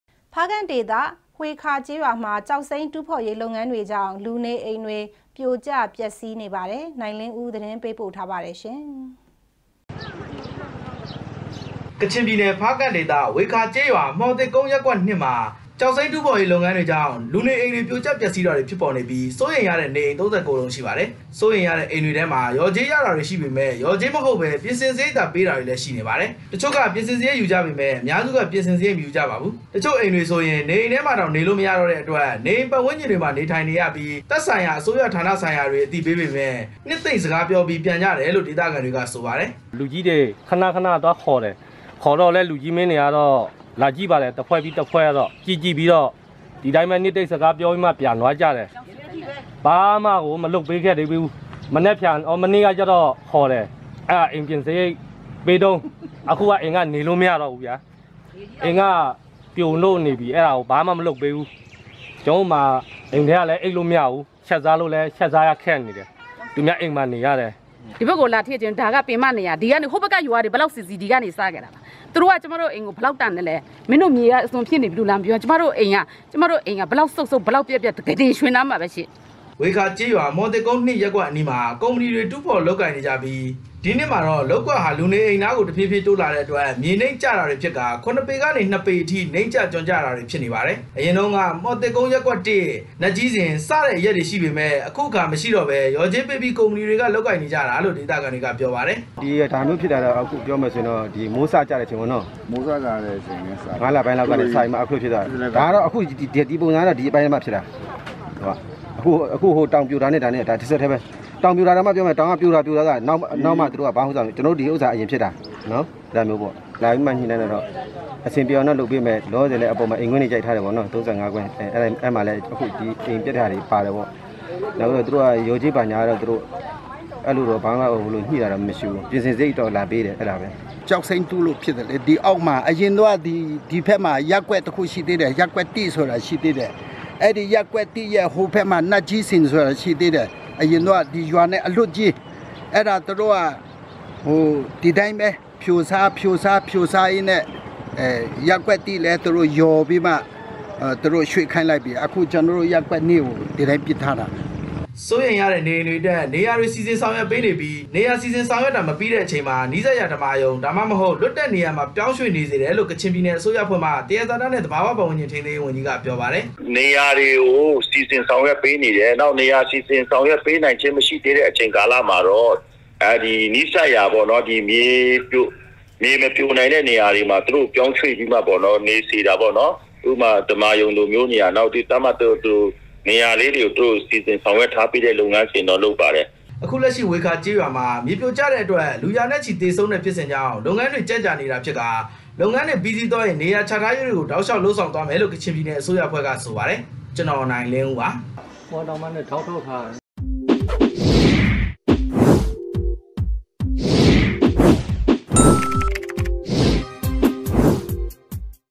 DVB - ကျောက်စိမ်းတူးဖော်ရေး လုပ်ငန်းတွေကြောင့် လူနေအိမ် ပြိုကျပျက်စီးတာတွေ ဖြစ်ပေါ်
ဖားကန့်ဒေသ ဝှေခါကျေးရွာမှာ ကျောက်စိမ်းတူးဖော်ရေး လုပ်ငန်းတွေကြောင့် လူနေအိမ် ပြိုကျပျက်စီးတာတွေ ဖြစ်ပေါ် နေပါတယ် ။ နိုင်လင်းဦး သတင်းပေးပို့ ထားတာပါ ။